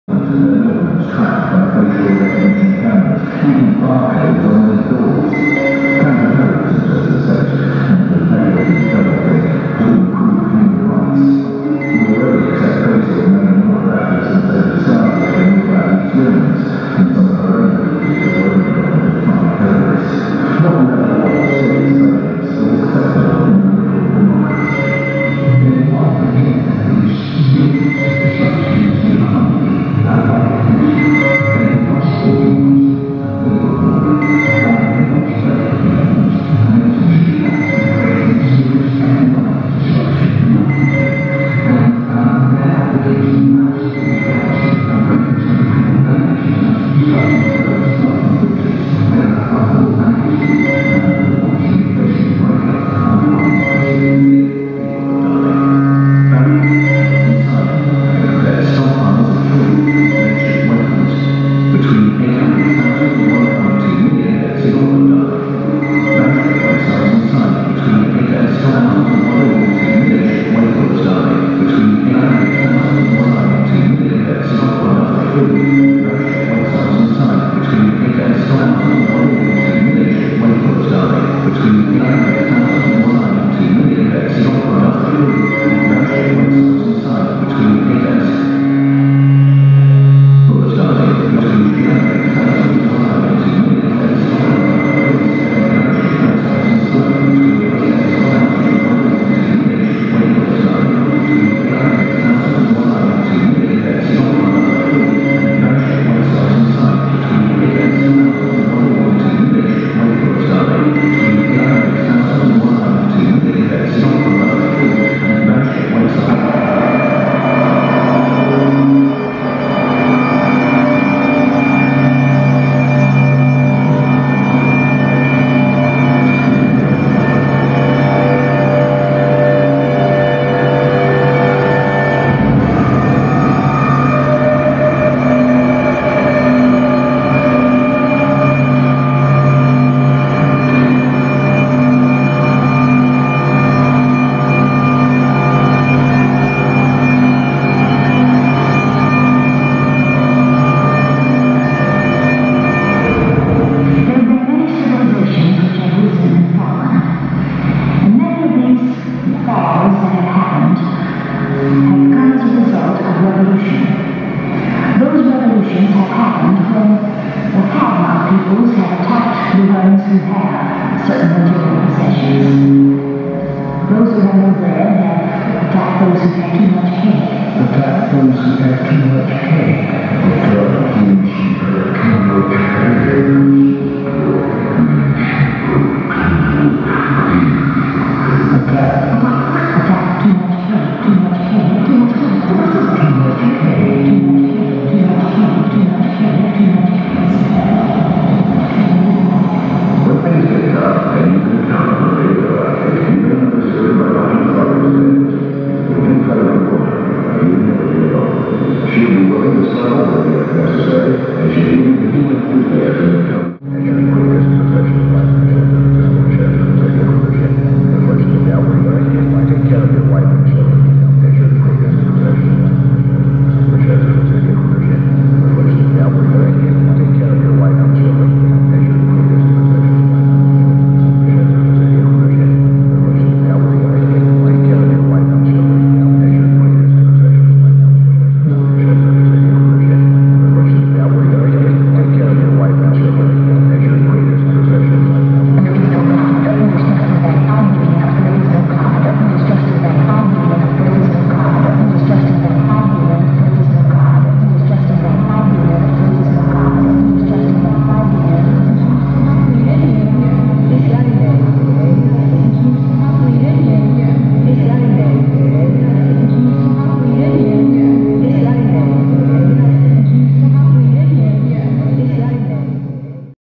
Terminal Optimism live at Weird Garden x-6 | x-church | Gainsborough | 21 Oct 2023
Terminal Optimism performing live at Weird Garden x-6, x-church, Gainsborough (21 October 2023).